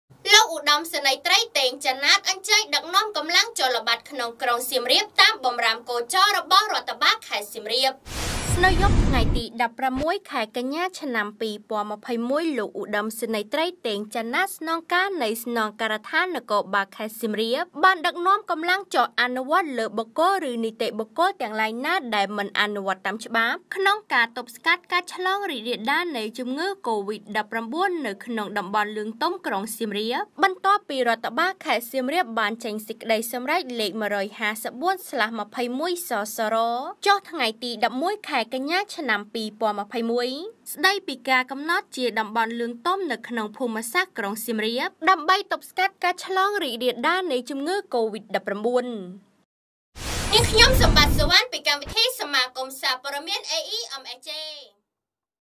លោកឧត្តមសេនីយ៍ត្រីតេងច័ន្ទណាតដឹកនាំកម្លាំងចុះល្បាតក្នុងក្រុងសៀមរាបតាមបំរាមគោចររបស់រដ្ឋបាលខេត្តសៀមរាប
លោកឧត្តមសេនីយ៍ត្រី តេង ច័ន្ទណាត អញ្ជេីញដឹកនាំកម្លាំងចុះល្បាតក្នុងក្រុងសៀមរាបតាមបំរាមគោចររបស់រដ្ឋបាលខេត្តសៀមរាប

សៀមរាប ៖ នៅយប់ថ្ងៃទី ១៦ ខែកញ្ញា ឆ្នាំ២០២១ លោកឧត្តមសេនីយ៍ត្រី តេង ច័ន្ទណាត ស្នងការនៃស្នងការដ្ឋាននគរបាលខេត្តសៀមរាប បានដឹកនាំកម្លាំង ចុះអនុវត្តលើបុគ្គល ឬនីតិបុគ្គលទាំងឡាយណា ដែលមិនអនុវត្តតាមច្បាប់ ក្នុងការទប់ស្កាត់ការឆ្លងរាលដាល នៃជំងឺ កូវីដ-១៩ នៅក្នុងតំបន់លឿងទុំក្រុងសៀមរាប បន្ទាប់ពីរដ្ឋបាលខេត្តសៀមរាប បានចេញសេចក្តីសម្រេចលេខ១៥៤/២១សសរ ចុះថ្ងៃទី១១ ខែកញ្ញា ឆ្នាំ២០២១ ស្តីពីការកំណត់ជា “តំបន់លឿងទុំ” នៅក្នុងភូមិសាស្ត្រក្រុងសៀមរាប ដើម្បីទប់ស្កាត់ការឆ្លងរាលដាលនៃជំងឺកូវីដ-១៩។